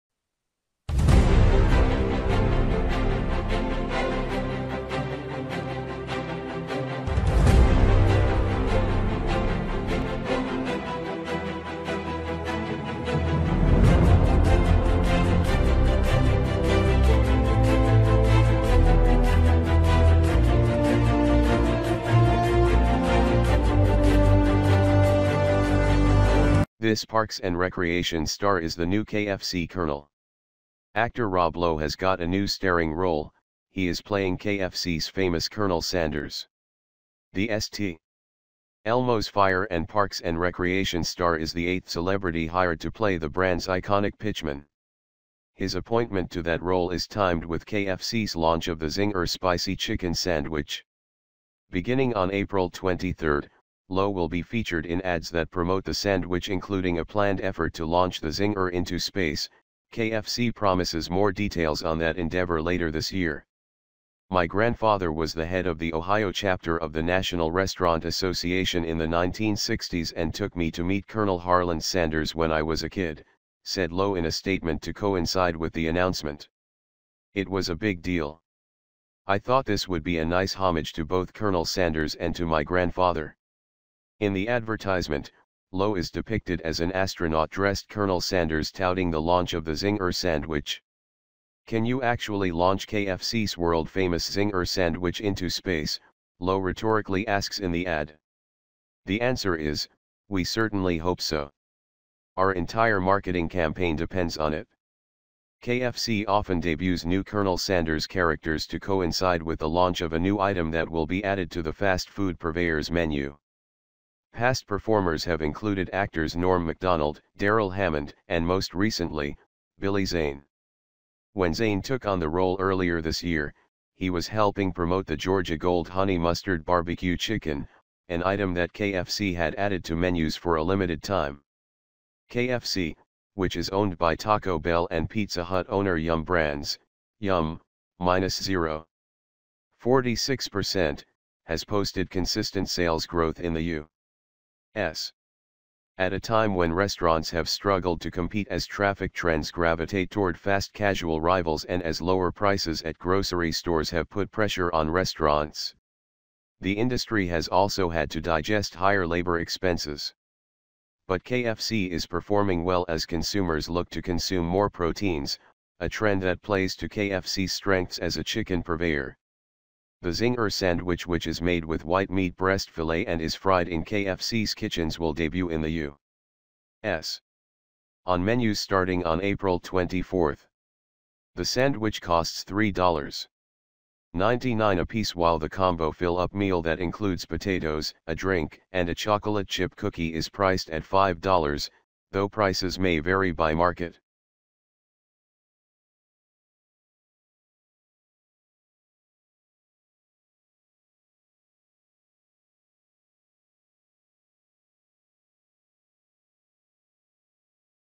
Who's the new KFC Colonel? West Winger Rob Lowe
Actor Rob Lowe has got a new staring role: he is playing KFC's famous Colonel Sanders.
The St. Elmo's Fire and Parks and Recreation star is the eighth celebrity hired to play the brand's iconic pitchman. His appointment to that role is timed with KFC's launch of the Zinger spicy chicken sandwich. Beginning on April 23, Lowe will be featured in ads that promote the sandwich—including a planned effort to launch the Zinger into space (KFC promises more details on that endeavor later this year).